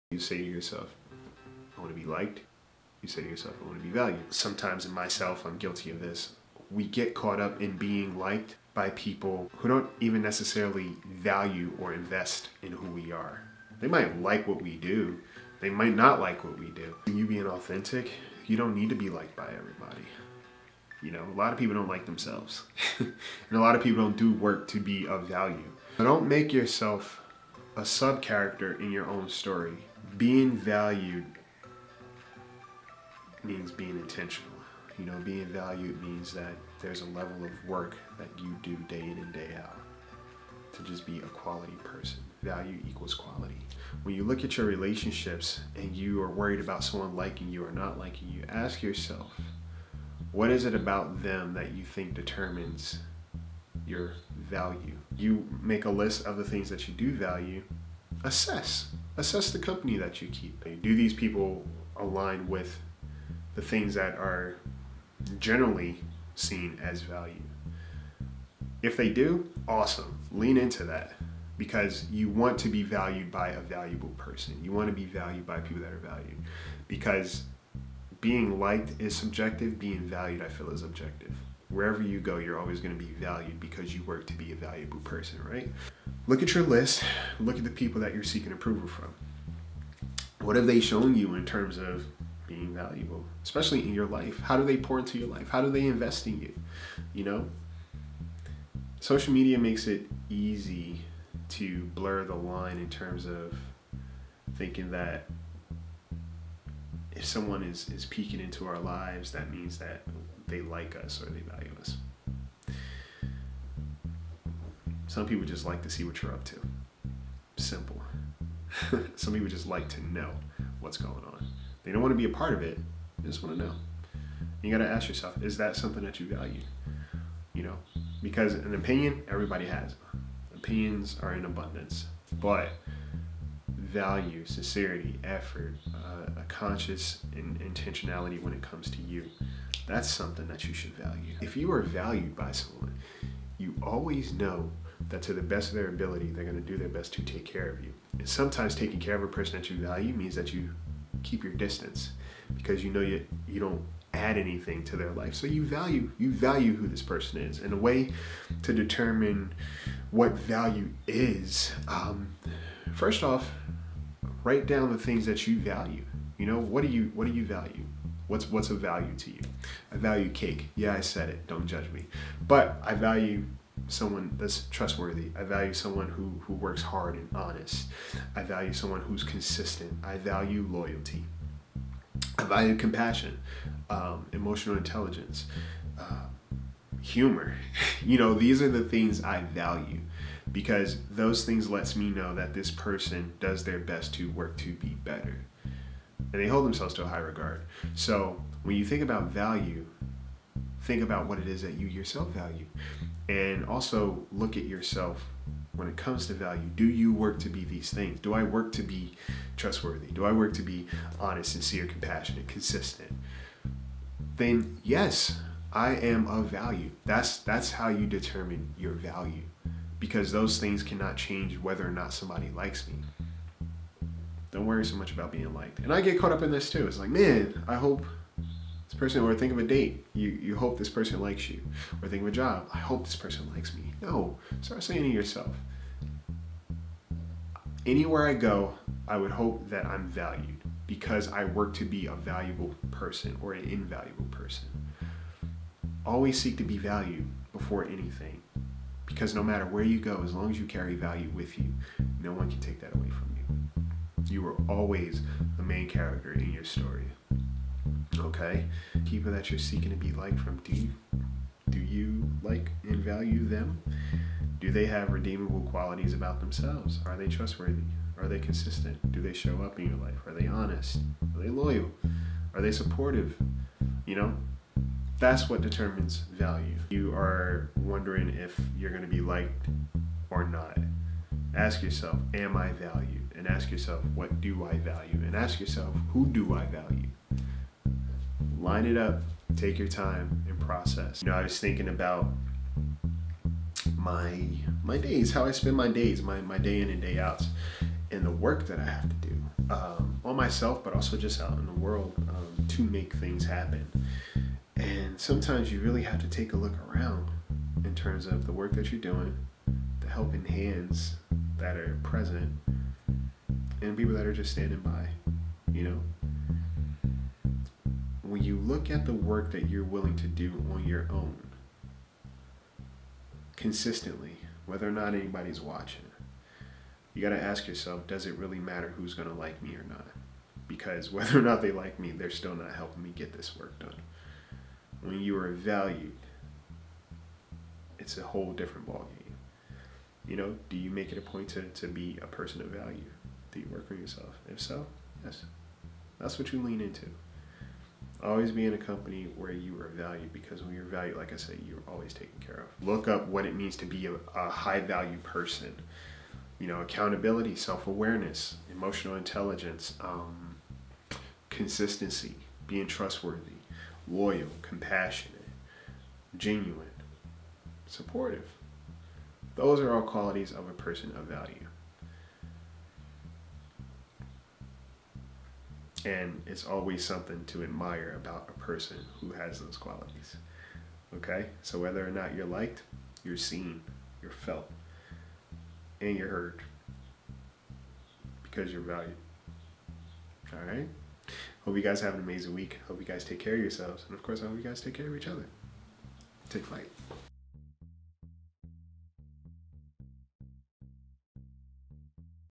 Bird Brain 128 Tell me what it's like to be valued
Today we talk about the difference between being liked and the power behind being valued.

Take Flight !